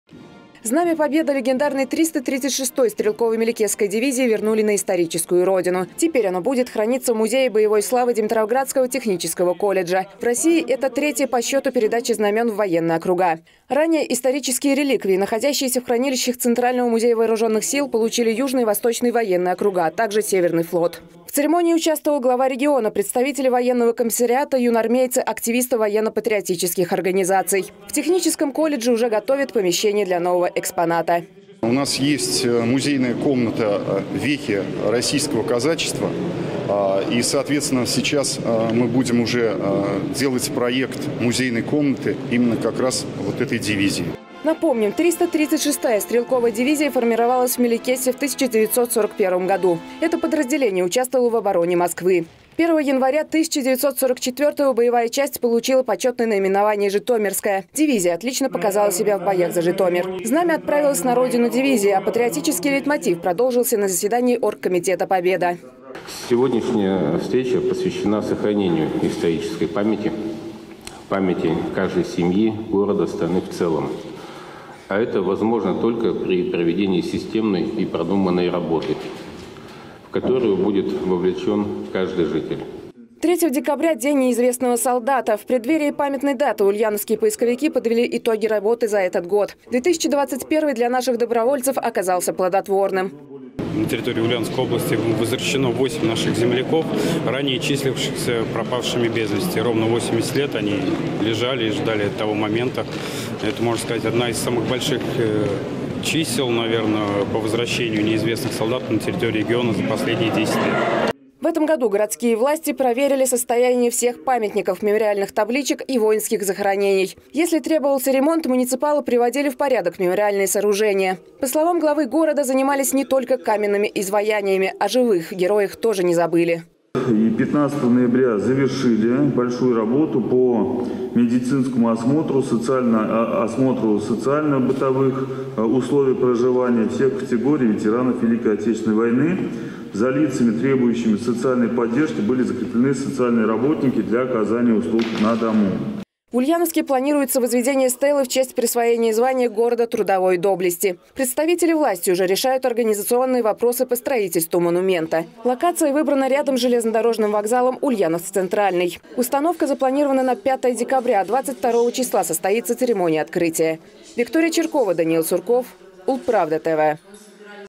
Воинам – слава, Димитровграду – знамя, Ульяновску - стела. Как в регионе память хранят
Год заканчивается, все структуры и сообщества подводят итоги проделанных работ. Немалую роль в развитии общества играет патриотическое воспитание. На заседании оргкомитета «Победа» власти поделились новостями и планами.